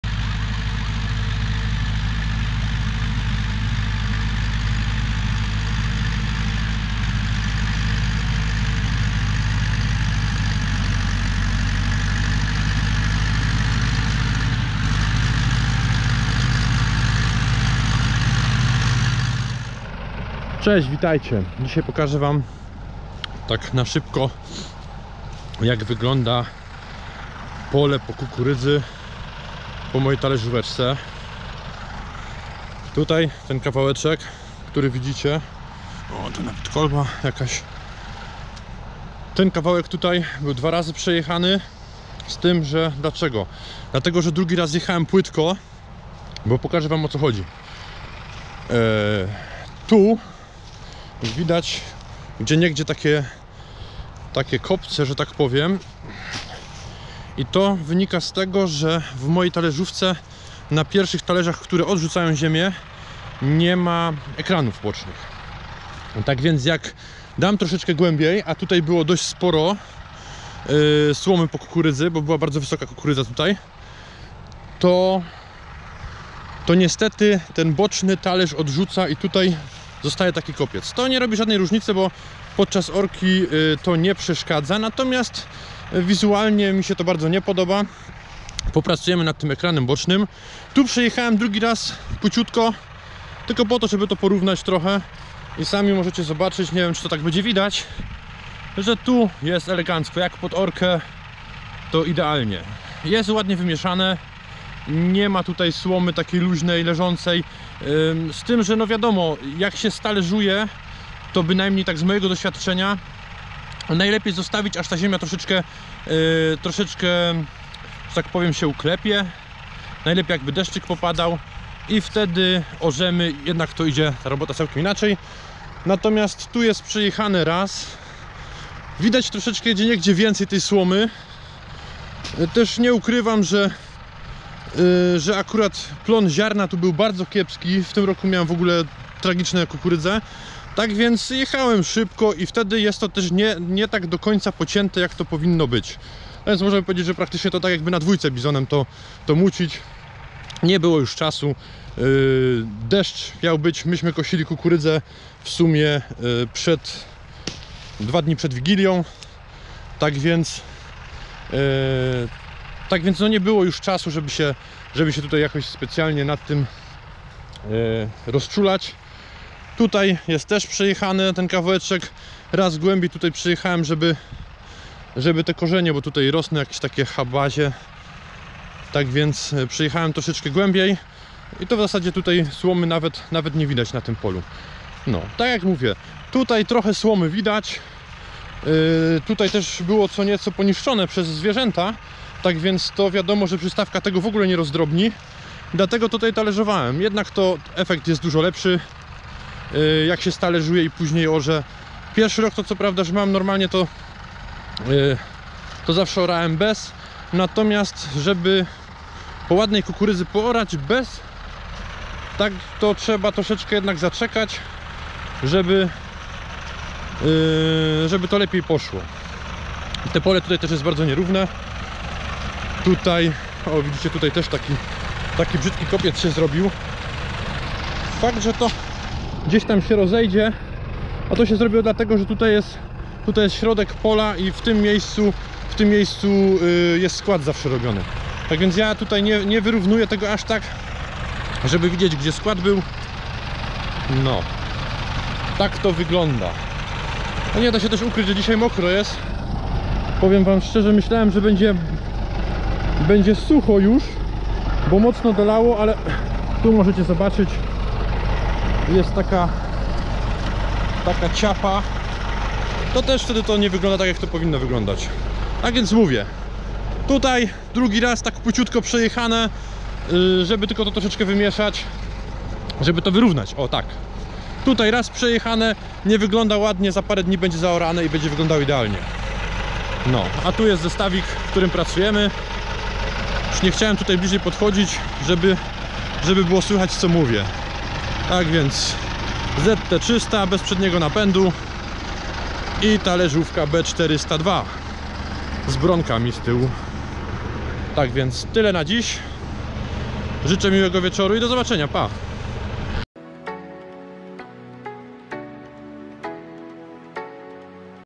Fortschritt B402 . Talerzowanie po kukurydzy. Jak to wygląda ......
Witam.
Filmik by pokazać jak wygląda pole po bronie b402 .